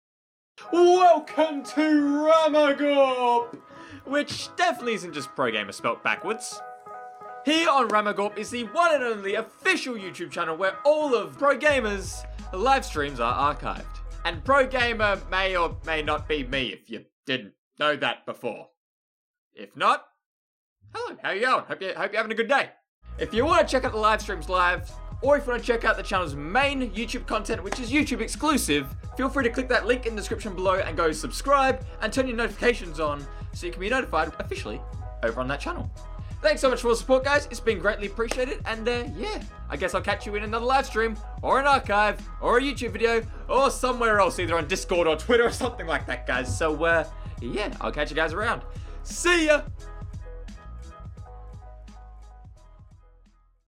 RAMMAGORP CHANNEL TRAILER!!!
Hey what’s going on guys, it’s your boy, ProGammar here and in today’s video we have an IMPORTANT channel update to make in regards to the archived livestreams on this channel. If you'd like to find out more about today's update, feel free to stick around to find out.

The Channel's Intro Was Made By Universe Archiblade. Below Is A Link To His YouTube Channel:

Think positive and think about the Pros over the Cons guys.